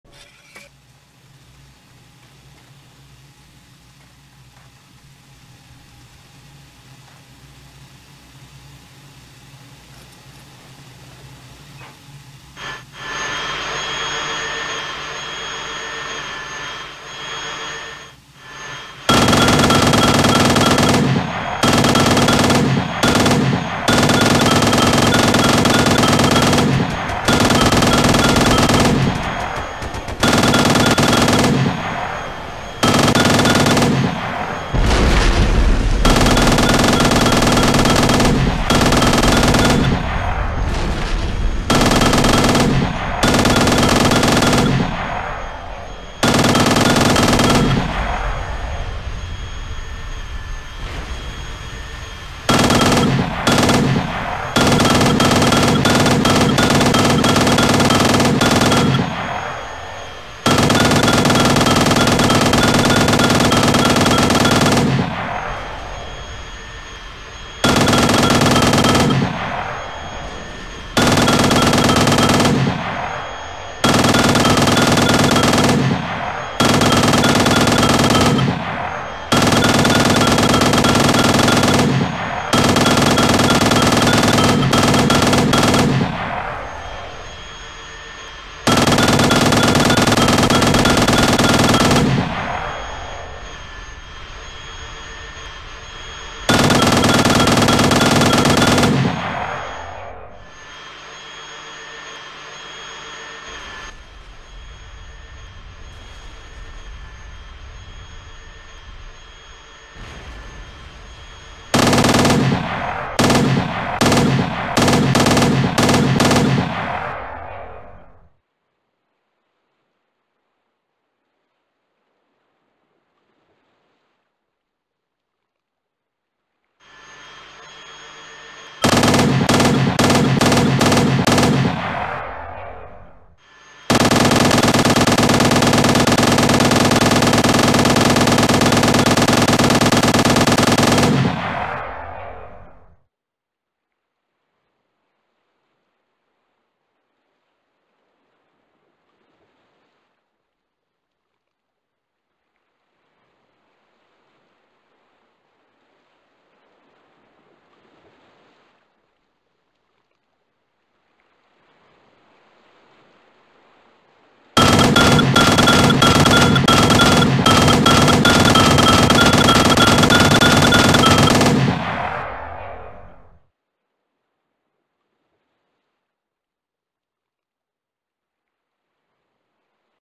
Defense of Redneck Island pt1
defending an island of biplanes. 21 were shot down in under 2 minutes